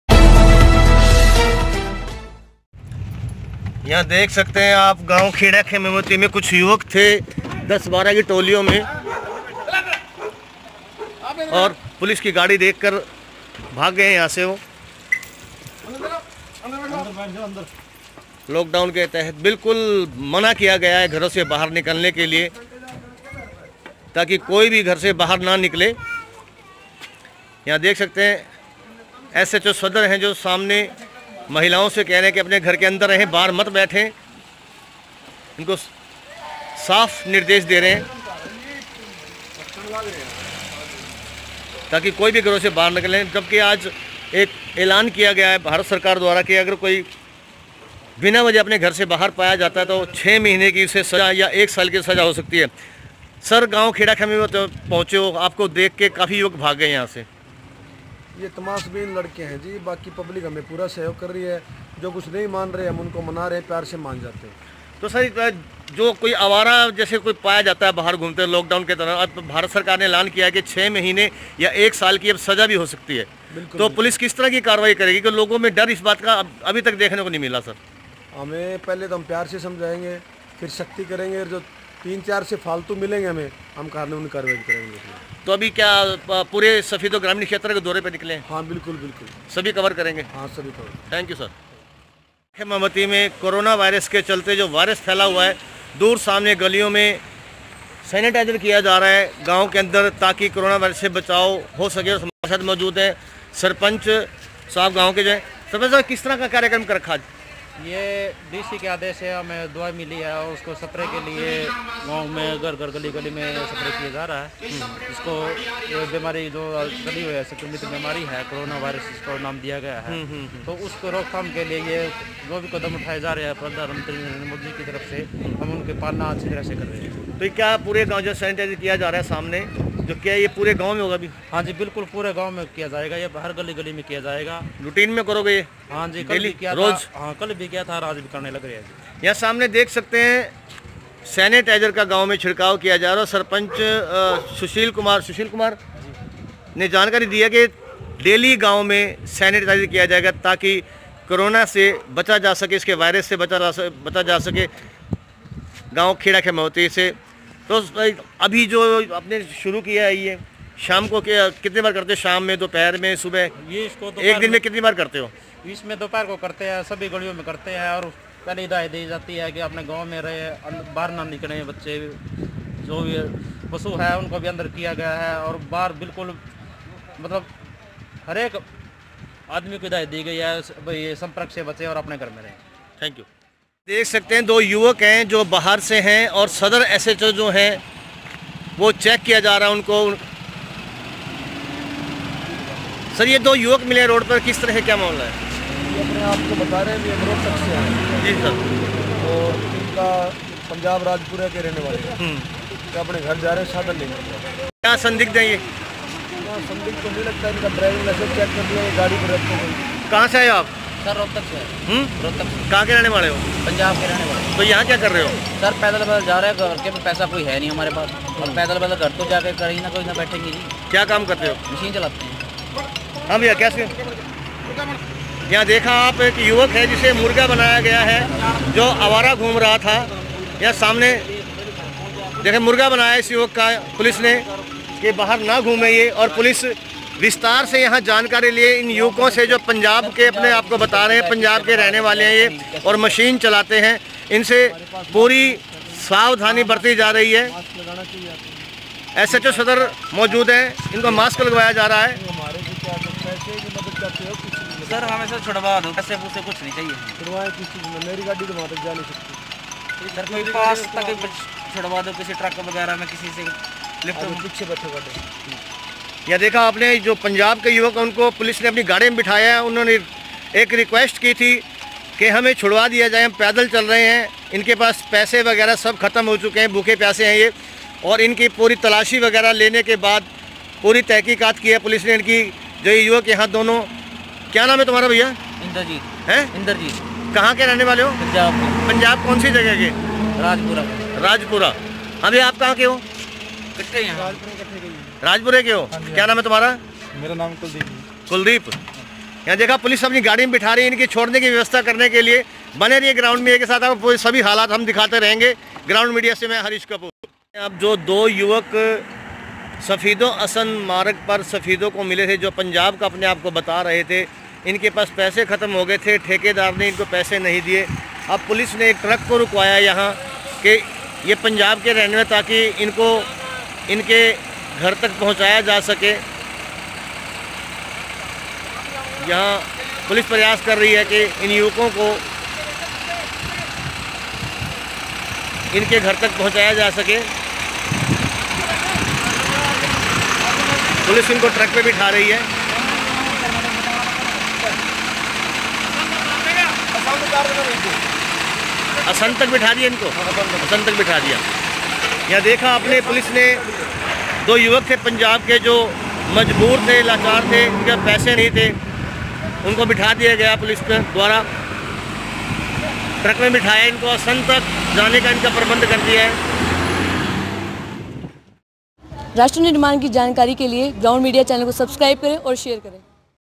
पुलिस के सिने में भी दिल होता है😢लाकडाउन में भूखे प्यासे दो युवकों की करी मदद🙏(देखें कैसे)
पुलिस के सिने में भी दिल होता है😢लाकडाउन में भूखे प्यासे दो युवकों की करी मदद🙏(देखें कैसे)Groundmedia